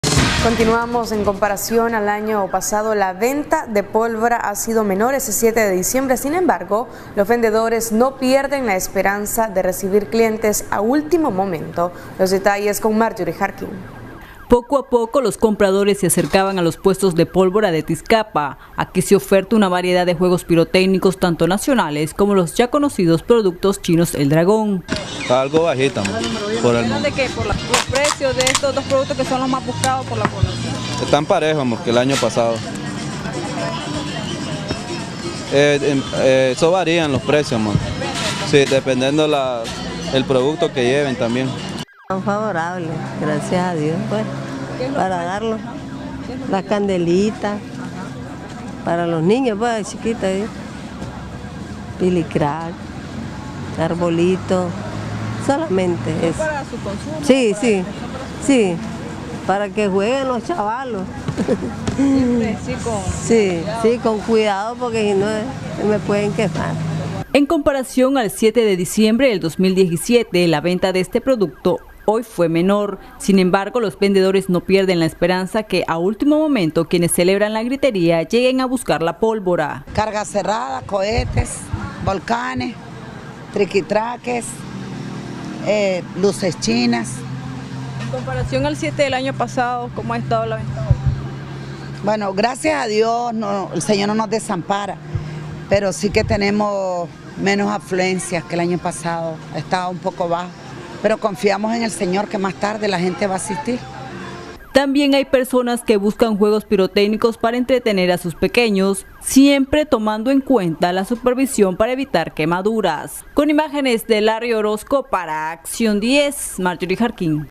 En comparación al año pasado la venta de pólvora ha sido menor
En comparación al año pasado la venta de pólvora ha sido menor este 7 de diciembre, sin embargo los vendedores no pierden la esperanza de recibir clientes a ultimo momento.